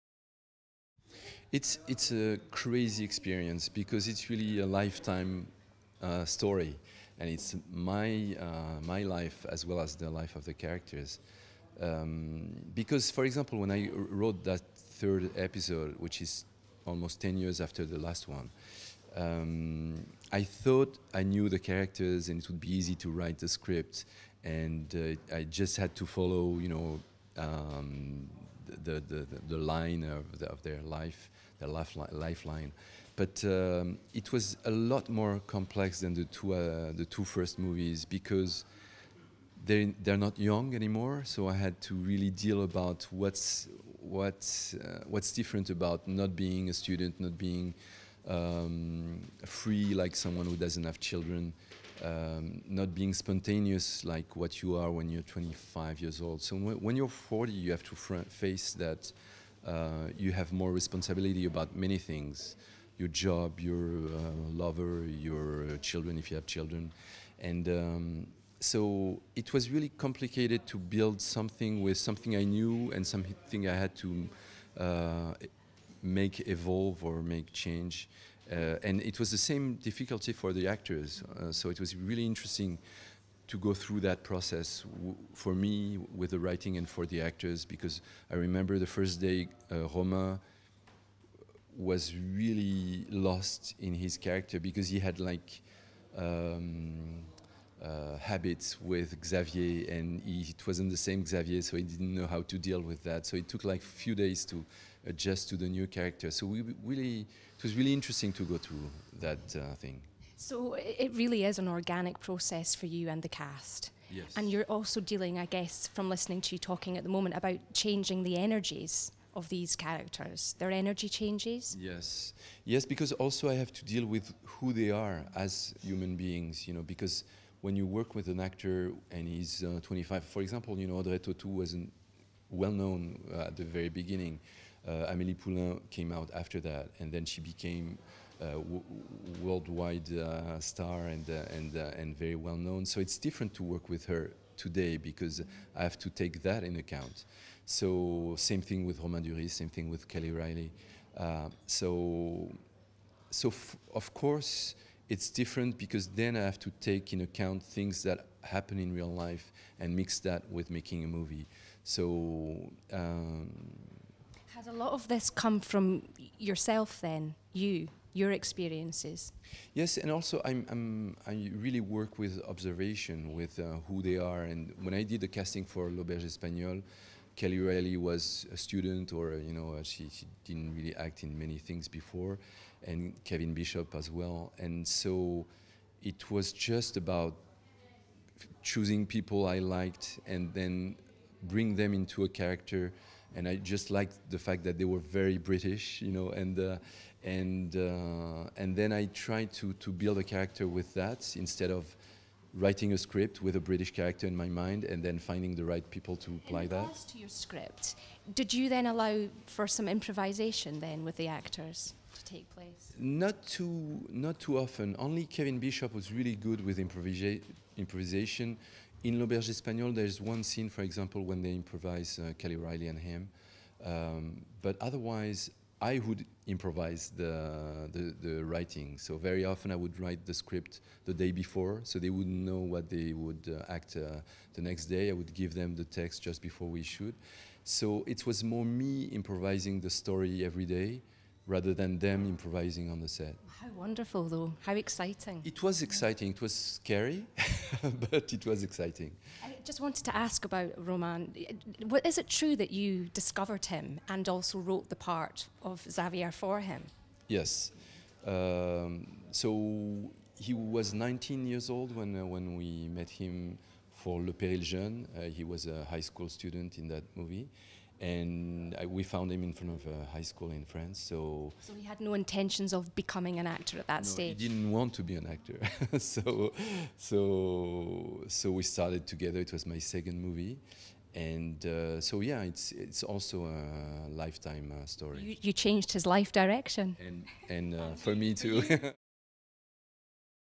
Chinese Puzzle Interview with Cédric Klapisch at the BFI LFF
At the BFI LFF Premiere Scene's Nicola Johnston and William McLaughlan had the pleasure of speaking to filmmaker Cédric Klapisch about is latest film Chinese Puzzle (Casse-tête chinois) which follows up  from Russian Dolls and Pot Luck (L'Auberge Espagnole). Join us as we ask Cédric Klapisch about his organic process of filmmaking and working with Audrey Tautou, Romain Duris and Kelly Reilly once more.

To see more interviews and to find out what's happening on the movie calendar visit